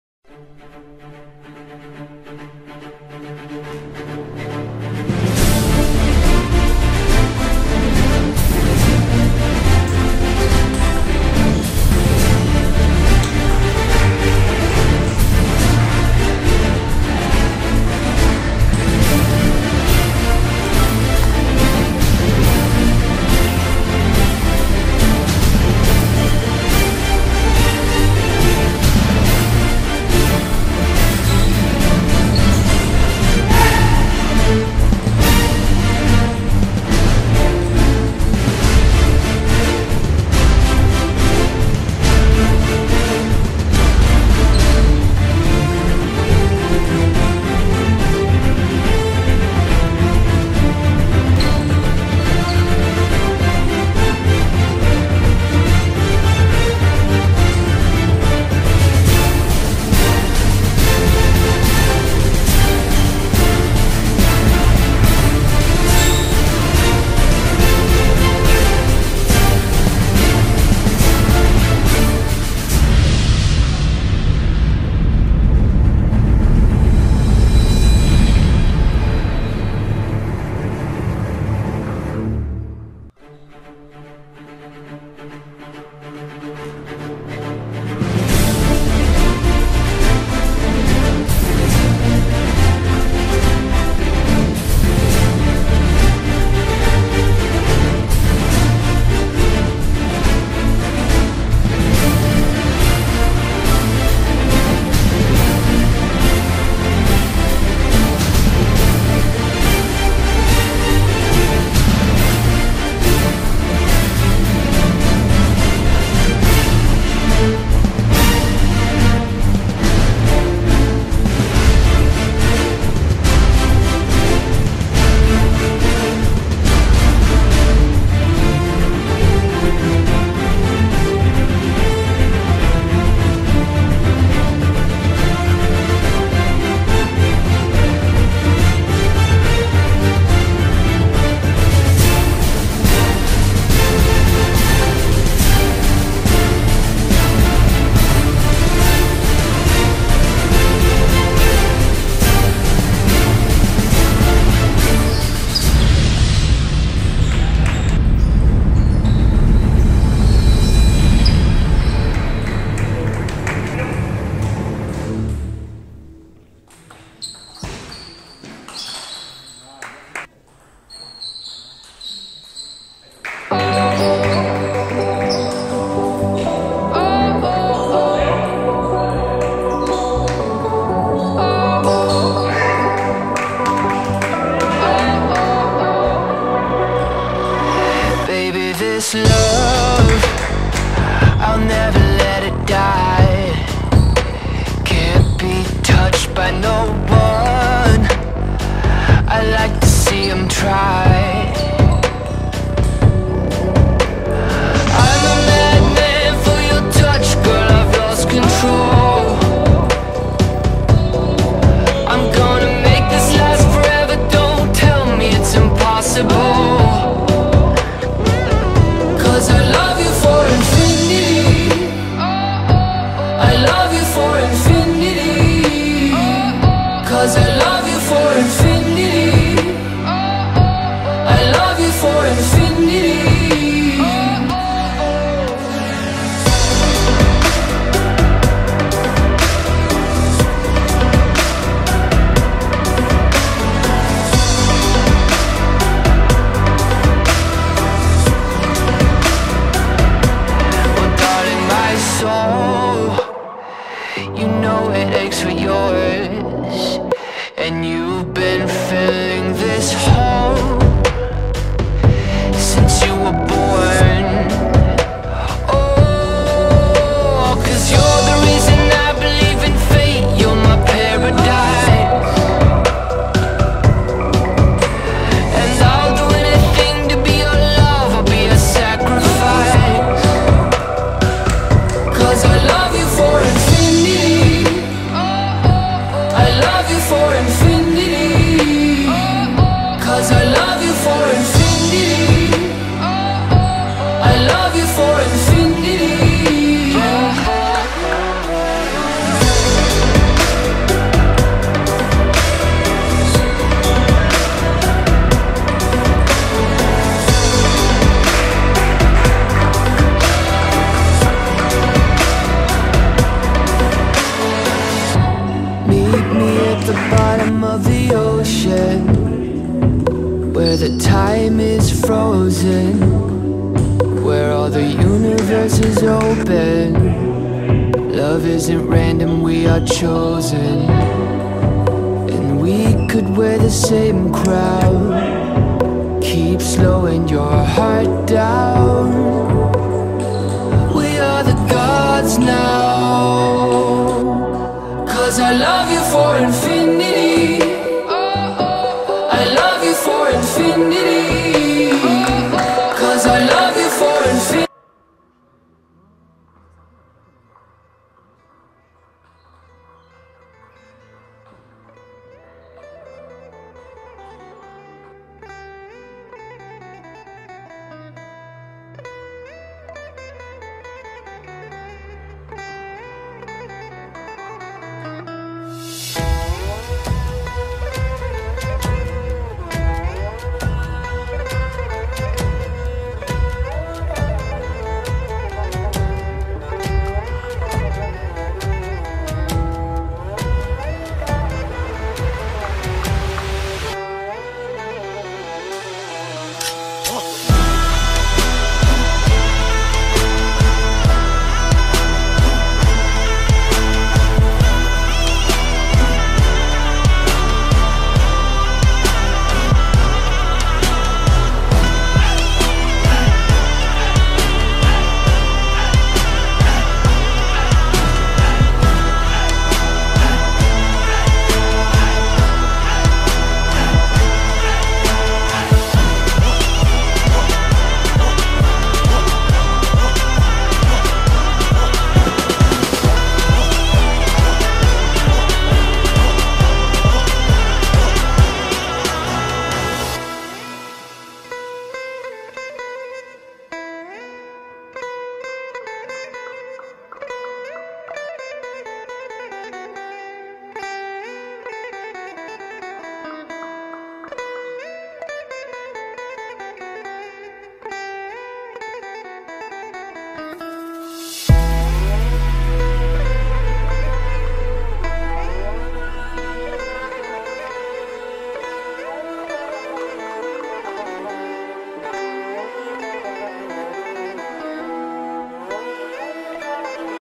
J11 Bègles - Perigné Pré-Nationale Tennis de Table
🏓 Best-of du match de l'équipe Fanion du C.A.Bèglais Tennis de Table.